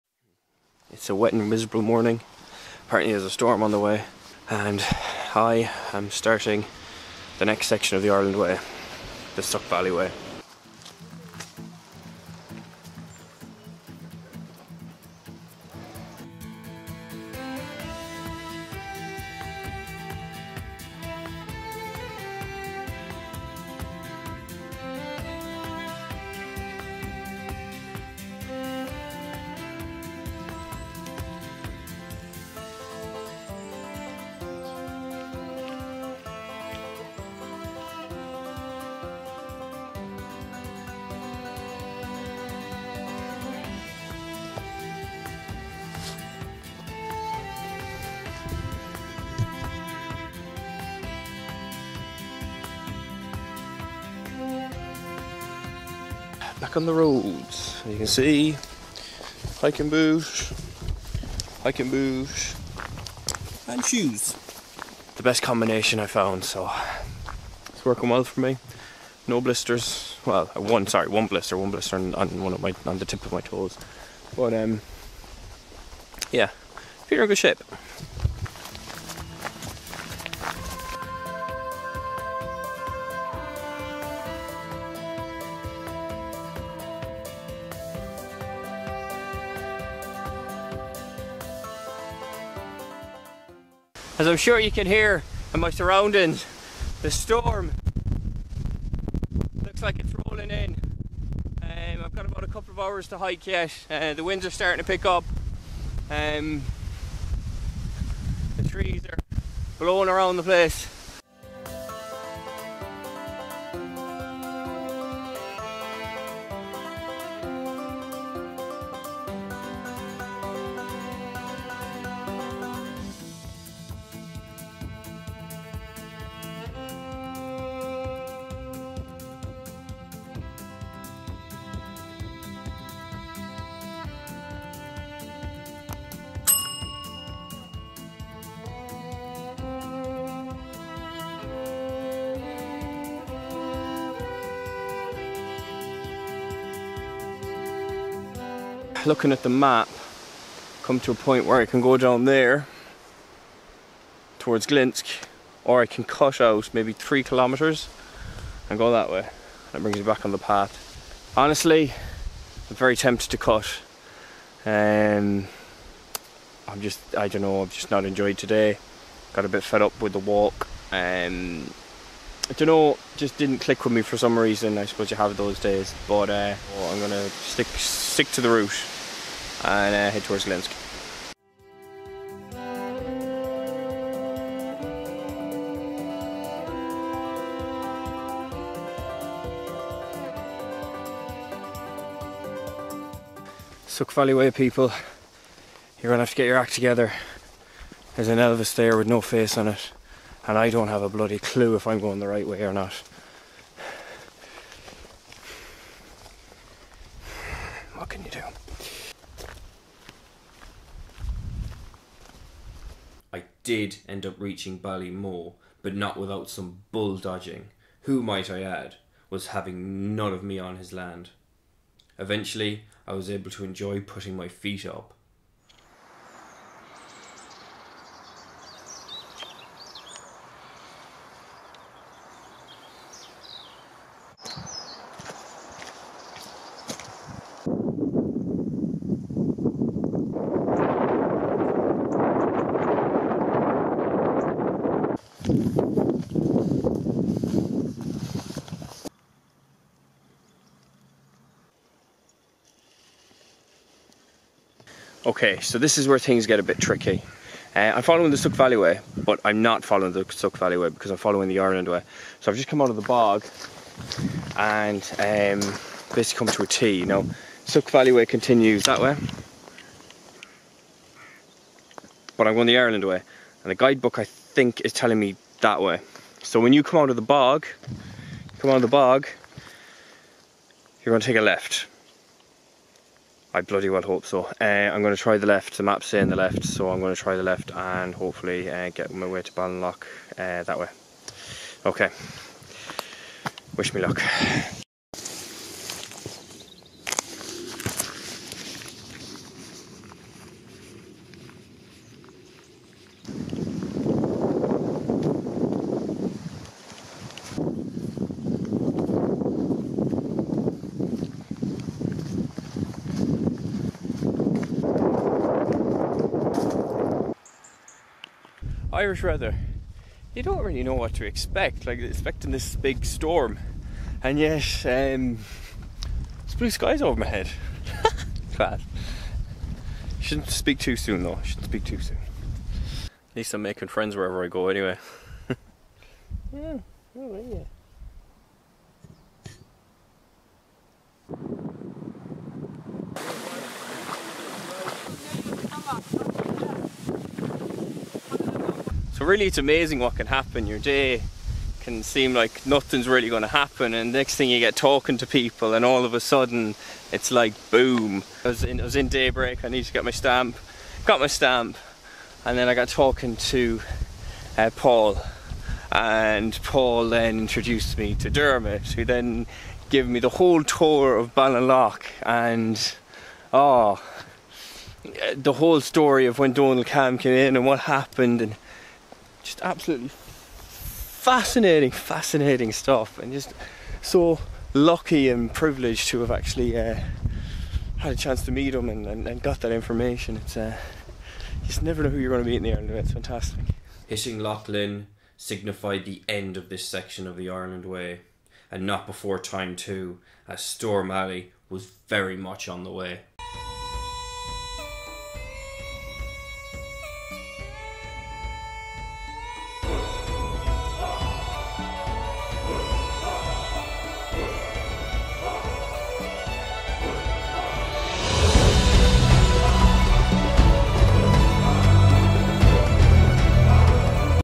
The Suck Valley Way (section 8) The Ireland Way
The Suck Valley Way is the 8th section of The Ireland Way.

This hiking trail takes in half of the original Suck Valley Way, a really lovely multi-day loop walk nestled in the Hidden Heartlands between Galway and Roscommon. You'll go through woods, past castle ruins, along fields and country lanes while dodging the odd bull.

During the two days I spent walking this section I was constantly watching for storm Ali who was on the way and edging ever nearer. Passing the halfway point of the enrite route I finished this section in Loughlynn.

I hope you enjoy this video please follow along and watch my epic adventure as I walk 1000km from the bottom all the way to the very top of Ireland